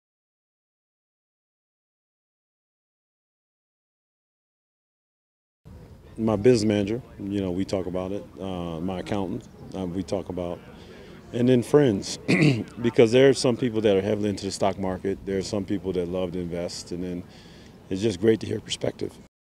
Ray Allen on Financial Perspective
Who does former NBA All-Star Guard Ray Allen rely on for financial advice?